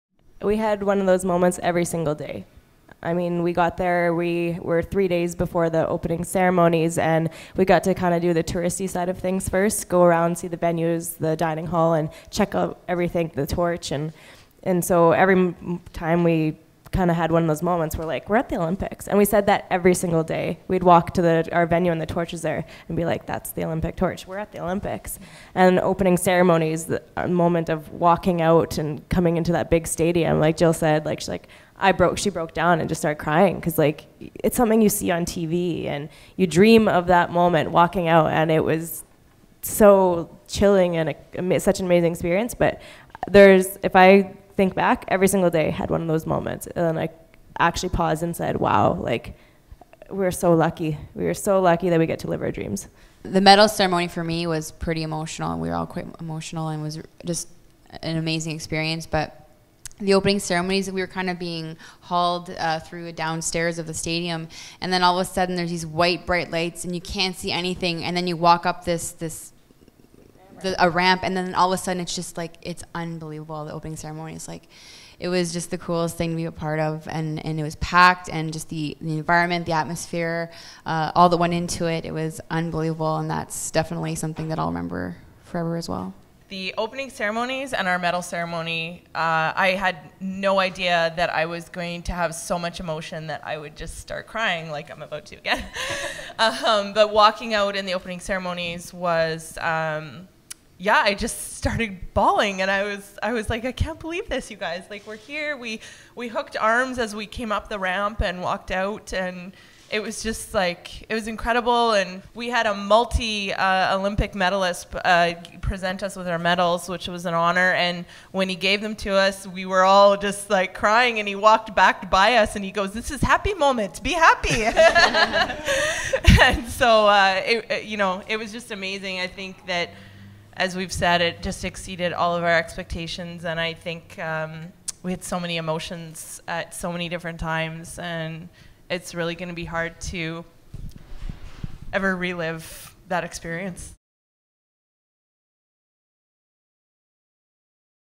Curling champs talk about their special Olympic moments
February 25, 2014 - Three of four members of Winnipeg's Team Jennifer Jones - Jill Officer, Kaitlyn Lawes and Dawn McEwen - talk about their special Olympic moments during a visit to the News Café.